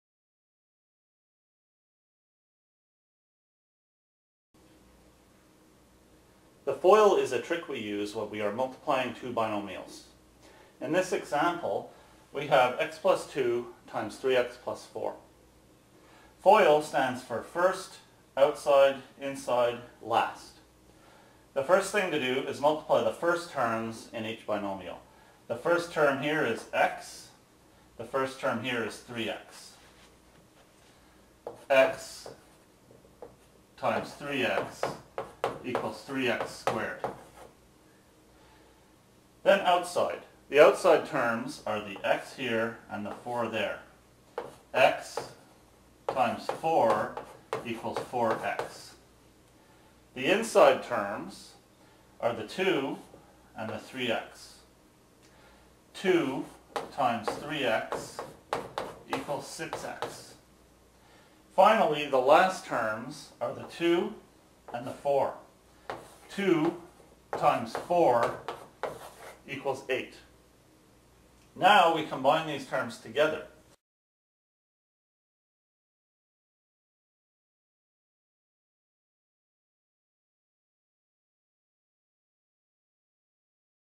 The FOIL - Multiplying 2 Binomials
This video illustrates a trick used when multiplying binomials. For part 2 of this video, as well as many moreinstructional Math videos and exercise and answer sheets, go to: